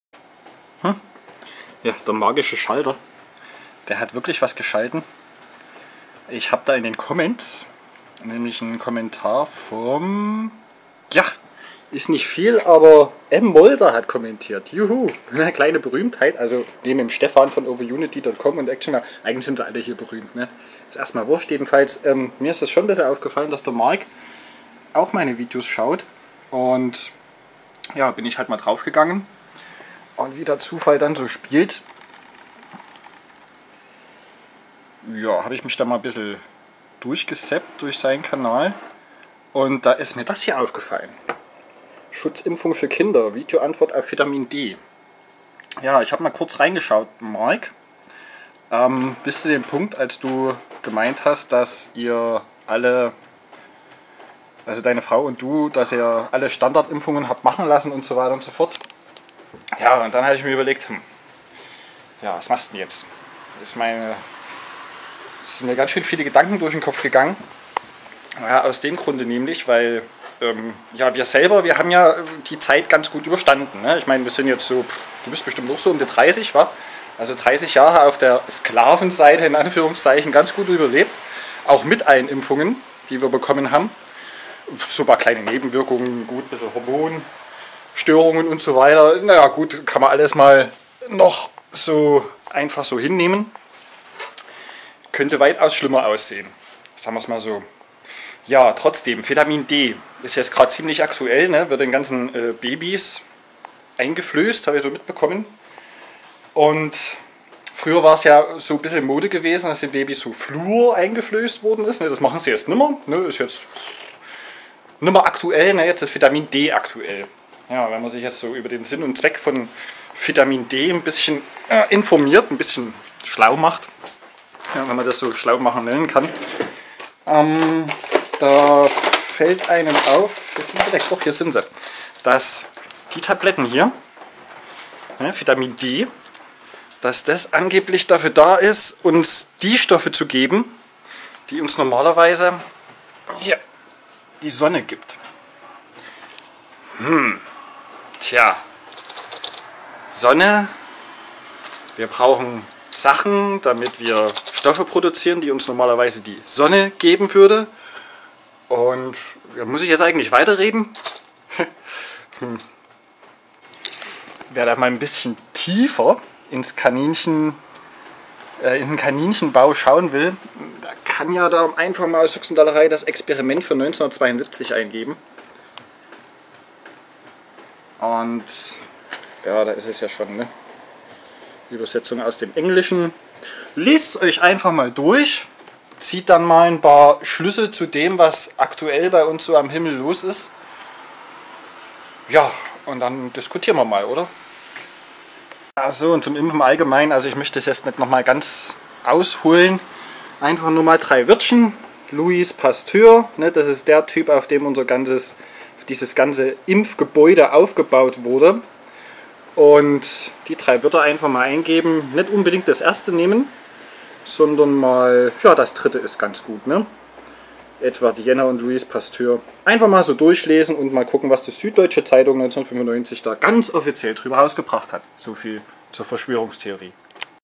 Wieso bekommen Babys Vitamin D?
Babys bekommen heutzutage keine Flourtabletten mehr sondern Vitamin D. Was steckt dahinter? Soll es etwa in Zukunft eine Menschheit geben die nicht mehr unbedingt Sonnenlicht benötigt?
Fakt ist zumindest: Das Sonnenlicht wird durch übermäßigen Flugzeugeinsatz stark abgeschwächt bevor es die Erdoberfläche erreicht. Auch wenn diese Chemtrails nur ein Schutz gegen etwaige Entladungen aus der Atmosphäre sind welche unsere Elektronik ausser Gefecht setzen könnte so ist es doch bemerkenswert, dass die fehlende Sonnenstrahlung durch Viaminpräparate aus der Pharmaindustrie gedeckt werden kann.
Wen es interesiert warum elektrische Entladungen so gefährlich sein können der möge sich die folgende Doku über das Plasmaversum anschauen

Und immer daran denken, dass dem normalem Sklaven das nicht gesagt wird. Er könnte womöglich durchdrehen und so seine Arbeit nicht mehr ausführen. Dies wäre wirtschaftlich ein schwerer Schlag ;)